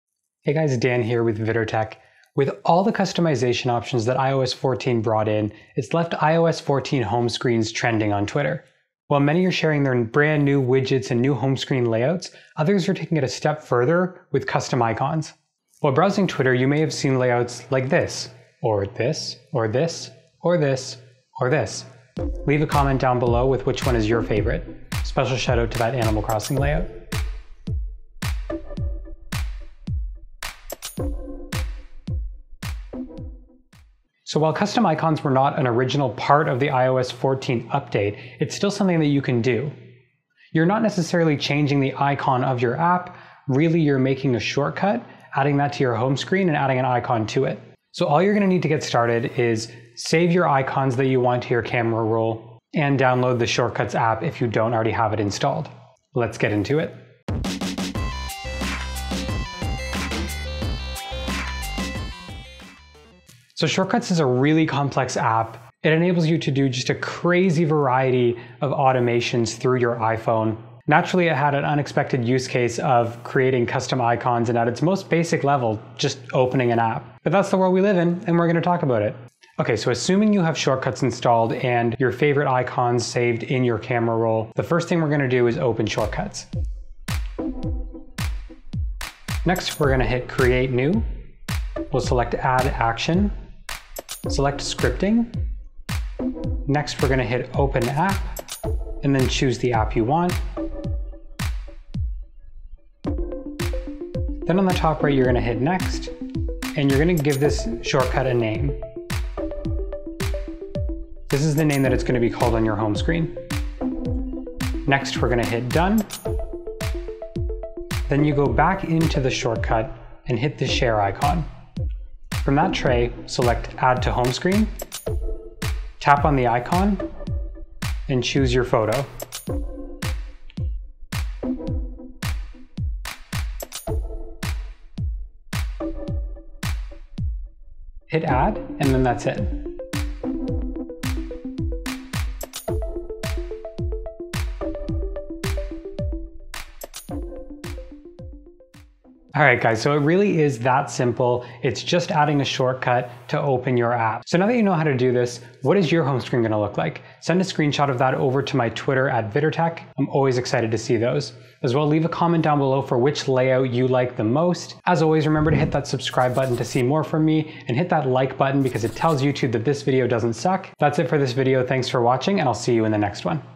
How to Make Custom Icons for iPhone (iOS 14)
With all of the customization options that iOS 14 brought to the table, it’s left iOS14Homescreens trending.

While many are showing off their new widgets and home screen layouts, others are taking it a step further with totally redesigned home screens with custom icons.

My Gear
Sony A7iii Camera

0:00 - Intro
0:17 - Home Screen Inspiration
1:19 - How to Make Custom Icons
2:30 - What will your home screen look like?